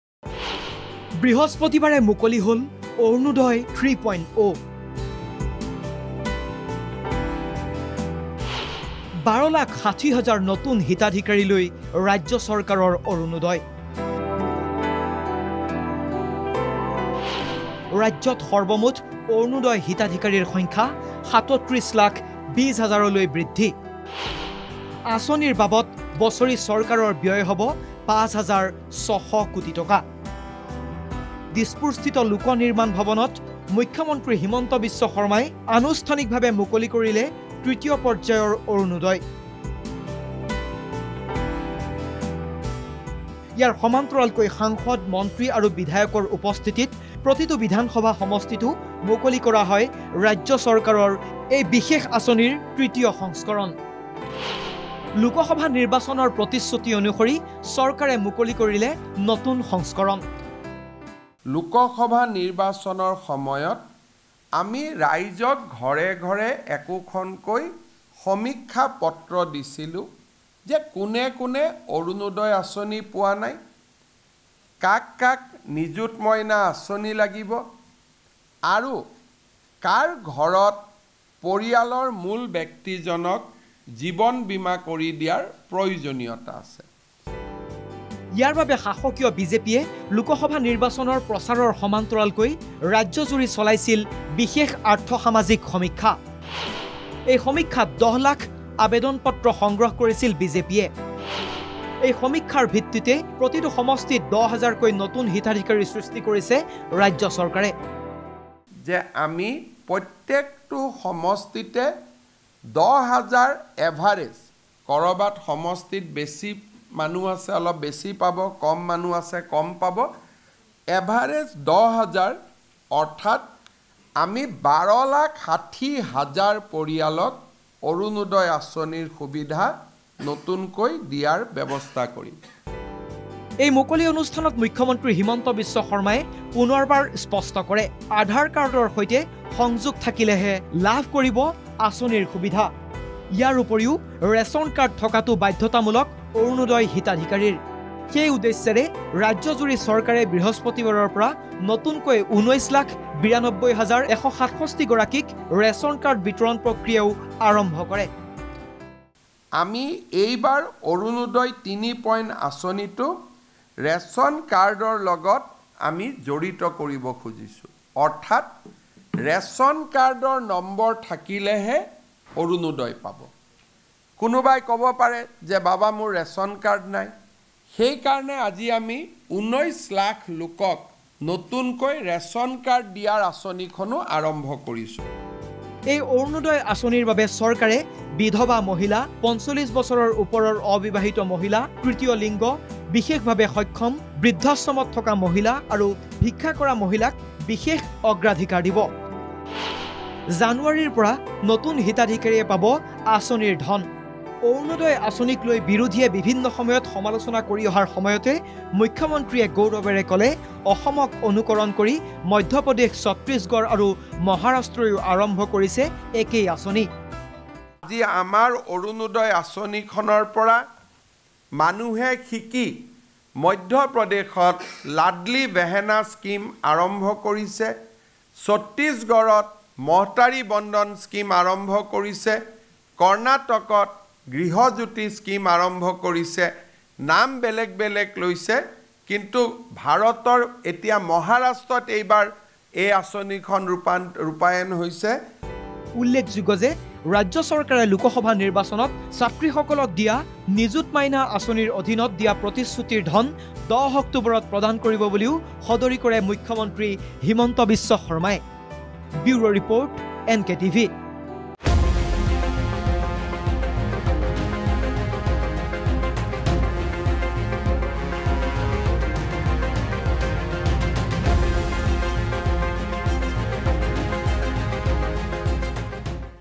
বৃহস্পতিবাৰে মুকলি হ'ল অৰুণোদয় ৩.০...
▶️ ১২ লাখ ৬০ হাজাৰ নতুন হিতাধিকাৰীলৈ ৰাজ্য চৰকাৰৰ অৰুণোদয়
▶️ ৰাজ্যত সৰ্বমুঠ অৰুণোদয় হিতাধিকাৰীৰ সংখ্যা ৩৭ লাখ ২০ হাজাৰলৈ বৃদ্ধি
▶️ আঁচনিৰ বাবদ বছৰি চৰকাৰৰ ব্যয় হ’ব ৫ হাজাৰ ৬ শ কোটি টকা